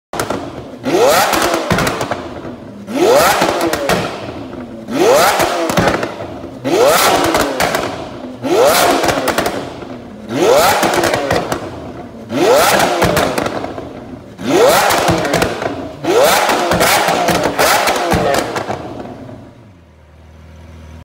Lamborghini Aventador S LP740-4 x Fi Exhaust - Insane Revs & Spitting Fire
Video Fitted with FI Complete Exhaust System includes:
Catless DownPipe.